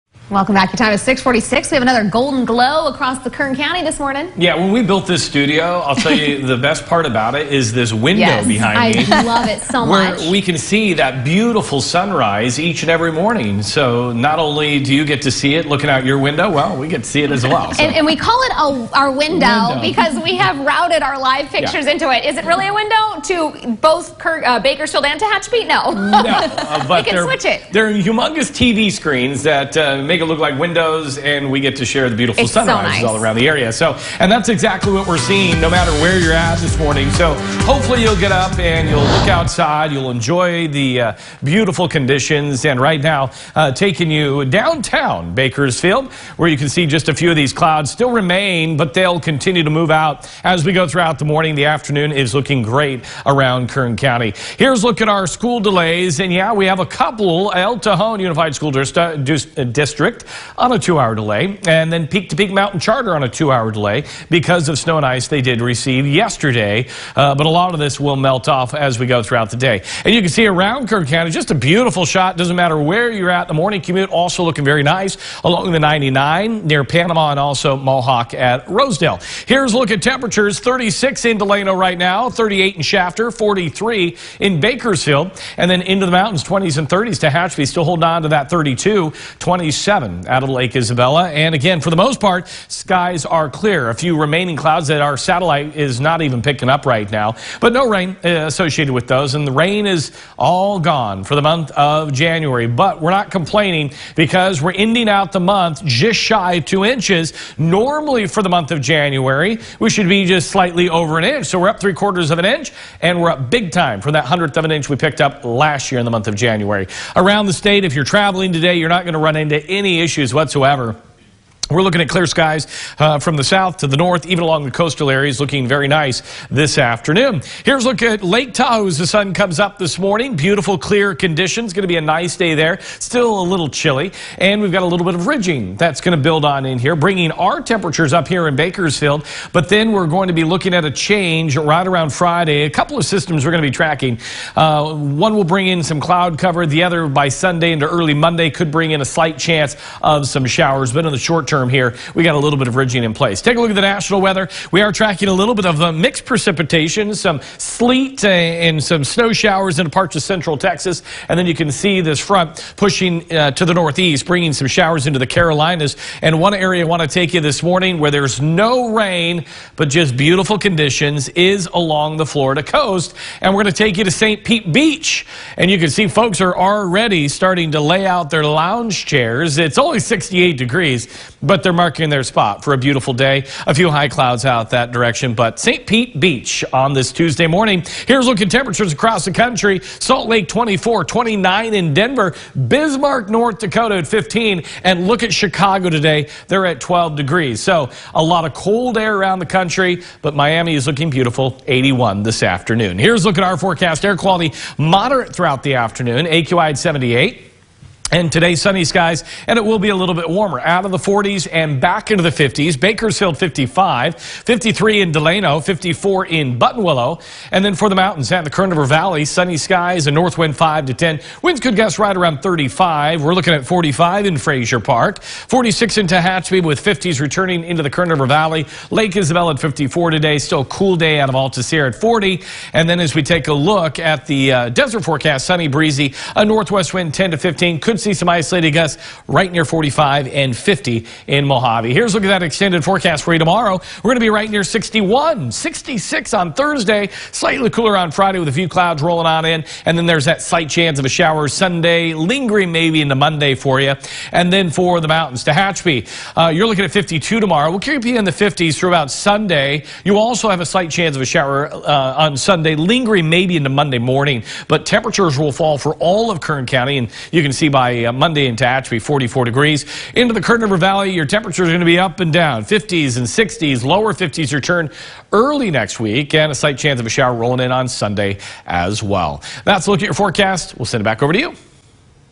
Expect warm and sunny skies, possible showers this weekend
The rain has moved out and sunny skies and warmer temps are in your forecast the next couple of days. We are tracking a few small storms that will track into California by the end of the week. The first storm will bring only clouds Friday, but the second will bring a slight chance of Showers Sunday into Monday.
We are ending out January with just under 2" of rain in Bakersfield.